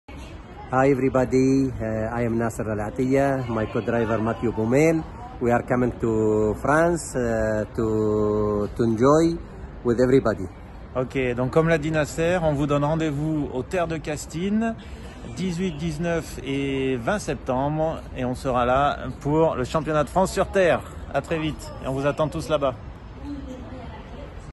Nasser al-Attiyah et Mathieu Baumel nous donnent rendezvous au Rallye Castine Terre d’Occitanie 2020
[Exclusivité] Nasser al-Attiyah et Mathieu Baumel nous donnent rendez-vous au Rallye Castine Terre d’Occitanie. Sur quelle auto ?!? 🤔
Pour les concurrents il est encore temps de s’inscrire !